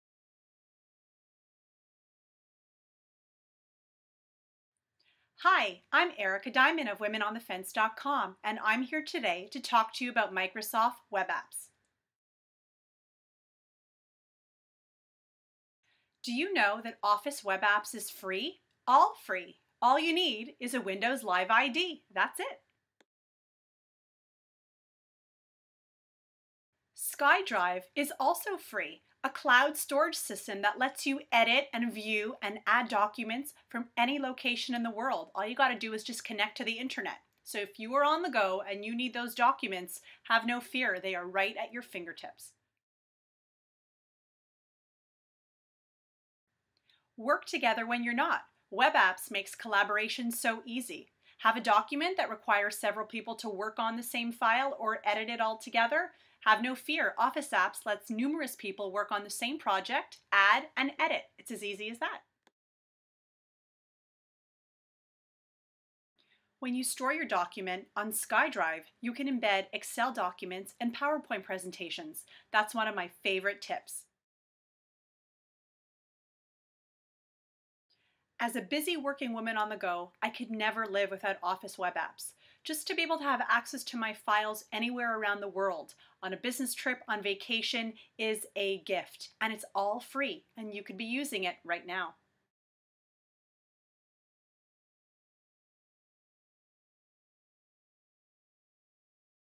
Microsoft Office Web Apps Features
Erica Diamond of spokesperson for Microsoft Office 2010 talks about all the great new features of Microsoft Office Web Apps